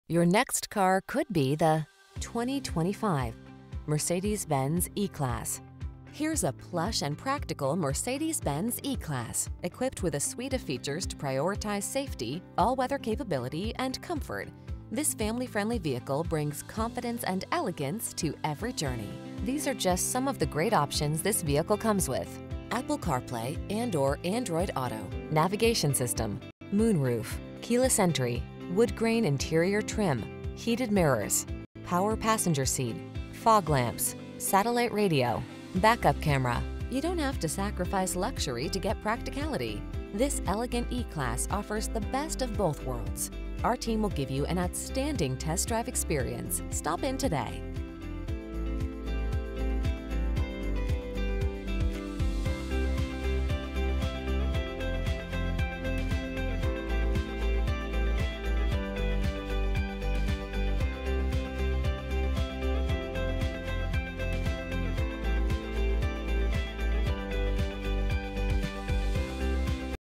2025 Mercedes-Benz E-Class CT Greenwich, Stamford, Darien, Port Chester, White Plains
New 2025 Mercedes-Benz E-Class E 350 available at Mercedes-Benz of Greenwich located in 261 West Putnam Ave, Greenwich, CT, 06830. Servicing the Greenwich, Stamford, Darien, Port Chester, White Plains area.
Please mention the stock SA117364 when you contact the dealership for this vehicle.